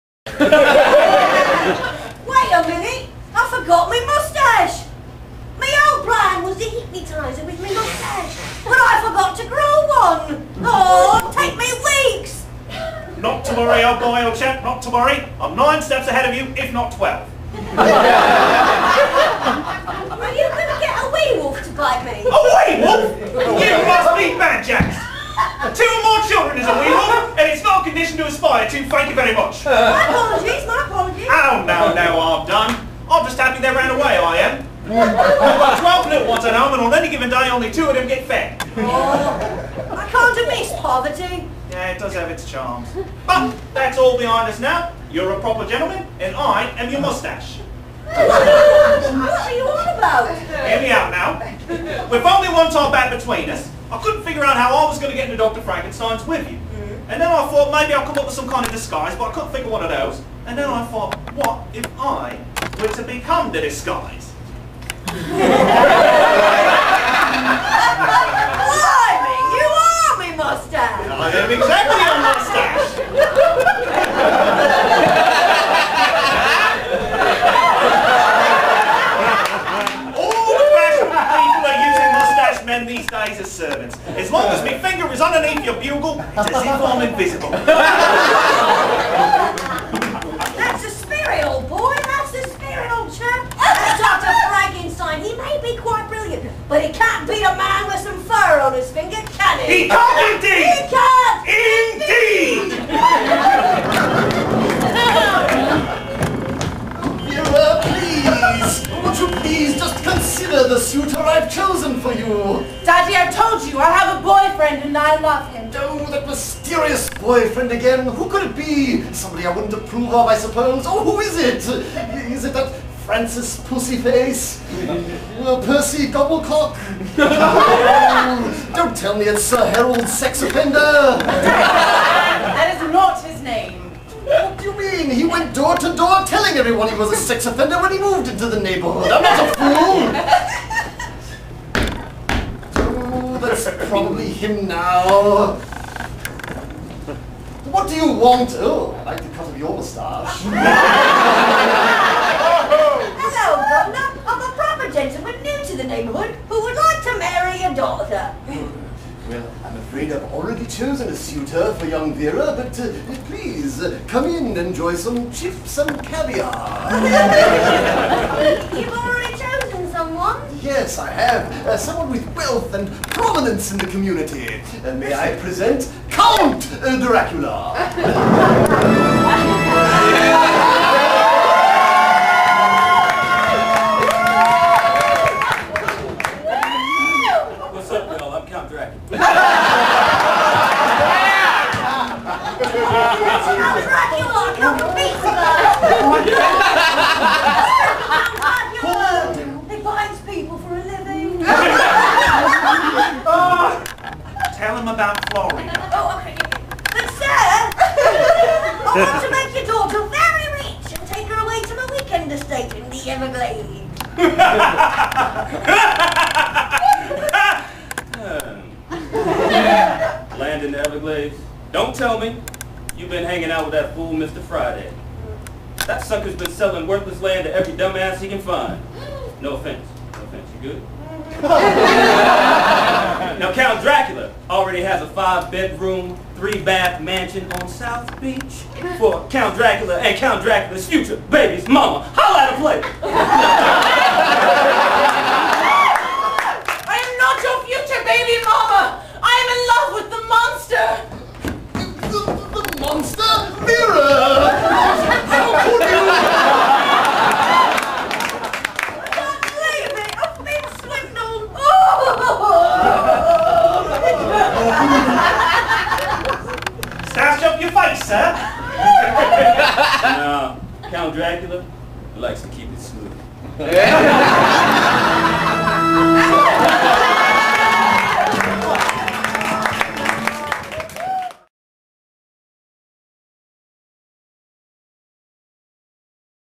SERIAL KILLERS: Grave Expectations, Ep. 3 (The 'Stache Gambit)
The gravediggers disguise themselves as gentlemen and court Dr. Frankenstein's daughter, Vera, as part of their plot to revenge themselves upon their former employer... but it transpires that the good doctor has already chosen Vera's suitor.

SERIAL KILLERS - Saturdays @ 11pm at Sacred Fools Theater Company in Los Angeles!  Five plays enter... Three plays leave!

Timmy: Heather Klinke
Tommy: Chad Fifer
Dr. Frankenstein: Eric Curtis Johnson
Vera: Mandi Moss
Count Draula: KJ Middlebrooks

Written & Directed by Chad Fifer